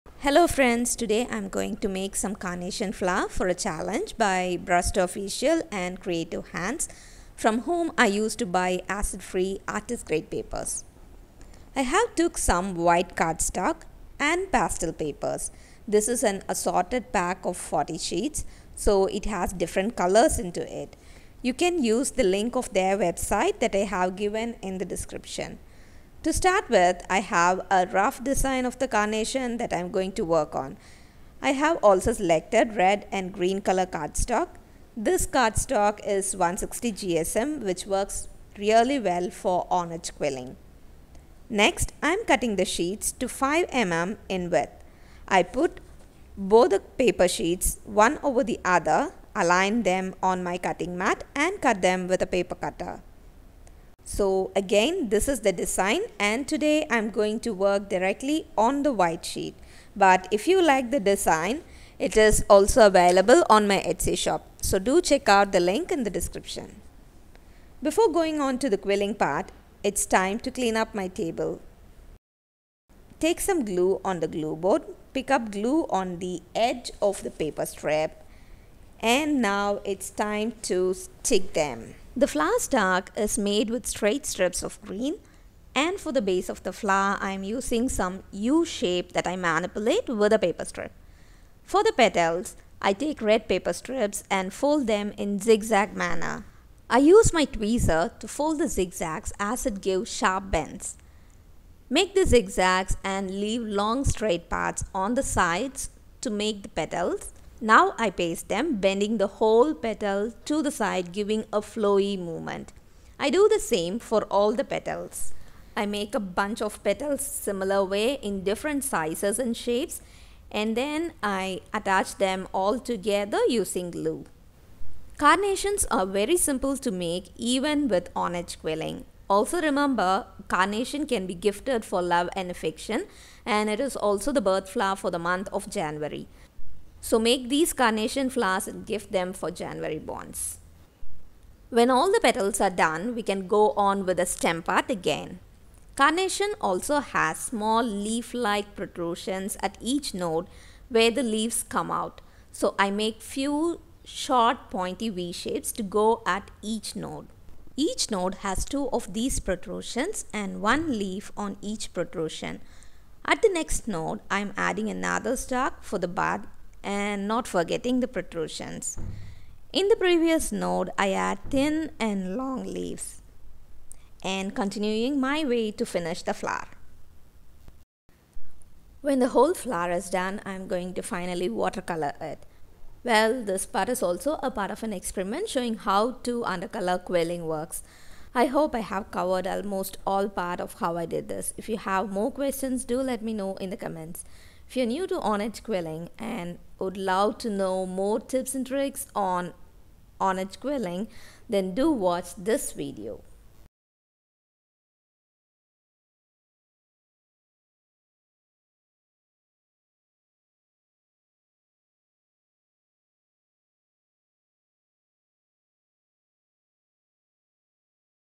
On-edge Quilling a Carnation flower | How to quill a Carnation flower
In today's video, let us make on -edge Quilling carnation flowers.

03:00 How to under color quilling works with watercolor

Craftgully (for quilling strips, quilling supplies and other craft supplies)

Amazon (for quilling tools and supplies)